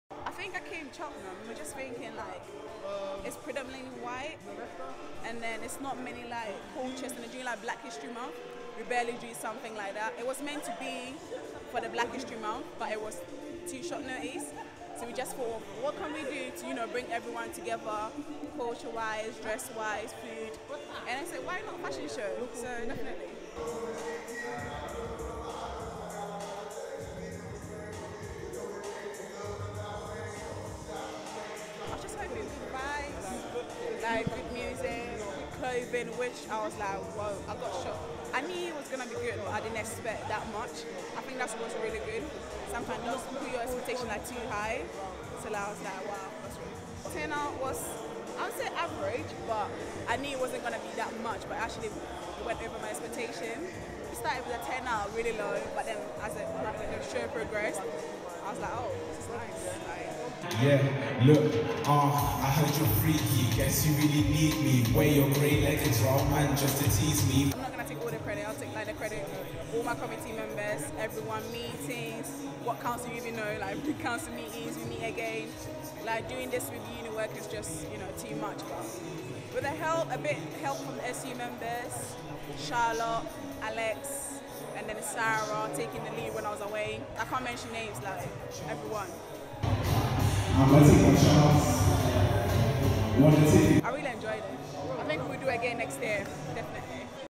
ACS Fashion Show - 5th April 2019
Interview with Angela Kodie, president of the Afro-Caribbean Society at UOG, about an event that they held at FCH chapel.